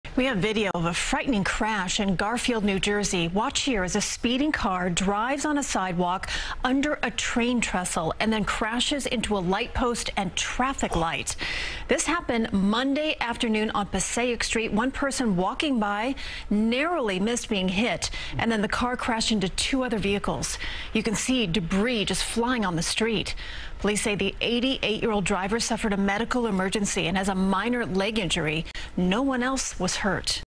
Video shows violent crash after driver suffered medical emergency in Garfield
Video shows a frightening crash in Garfield, New Jersey.